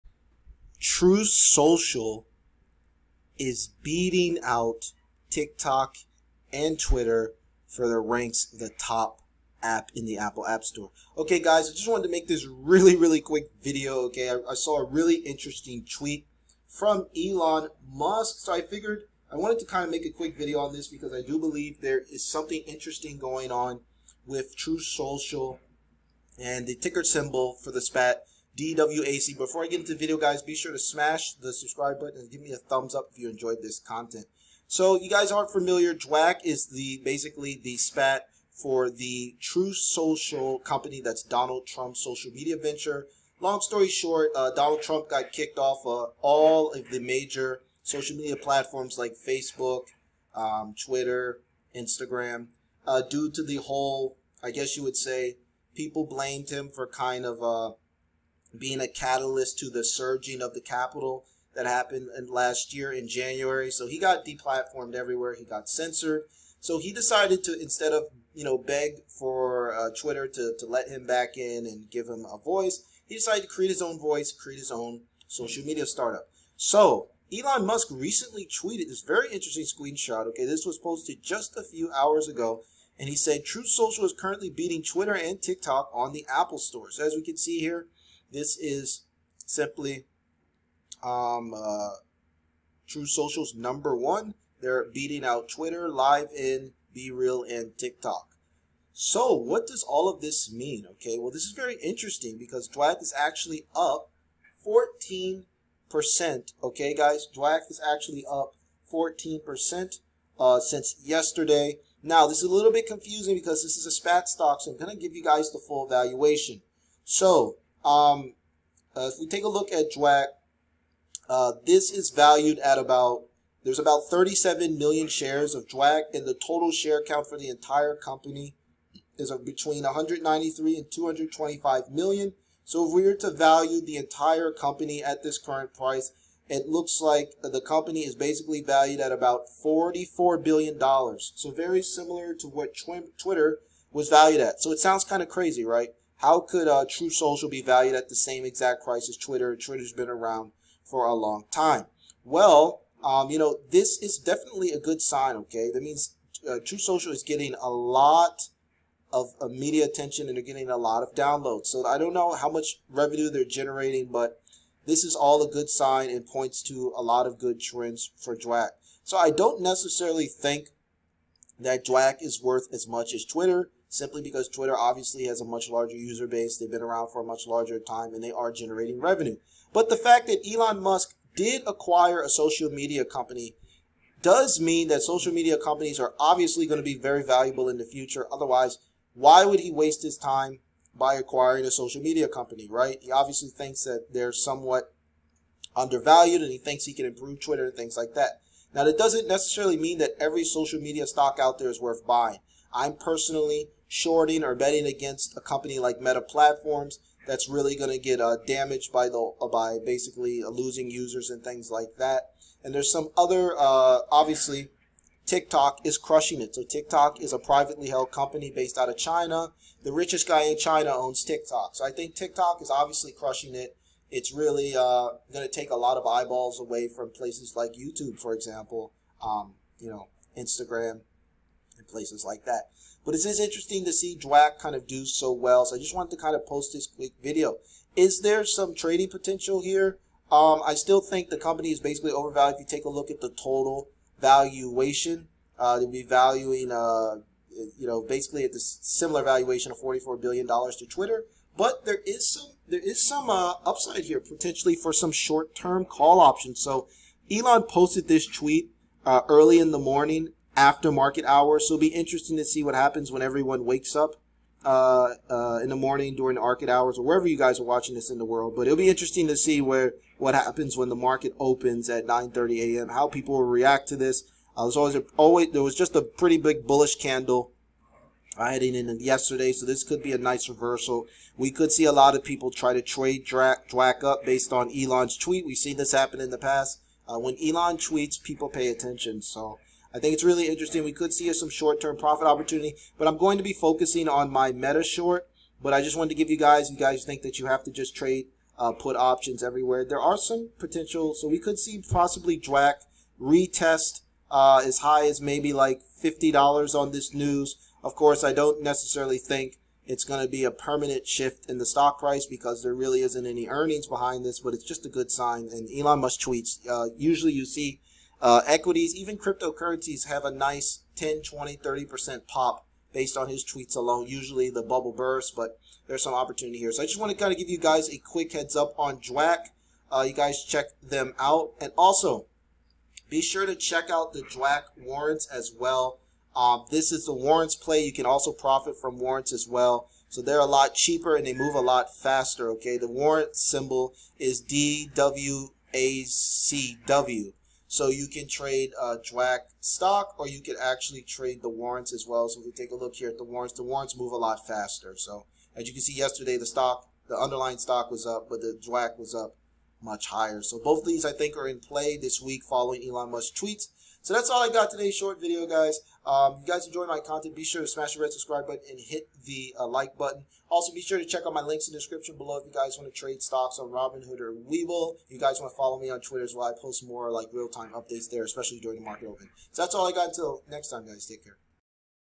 Truth Social App Ranks #1 on Apple Store! (Will DWAC Stock Soar After Elon Musk's Tweet?)
DWAC Stock May Soar After Elon Musk's Tweet about Truth Social App Ranking #1 in the Apple Store. Here's why DWAC Stock Could Jump 10 to 20% following Musk's Tweet.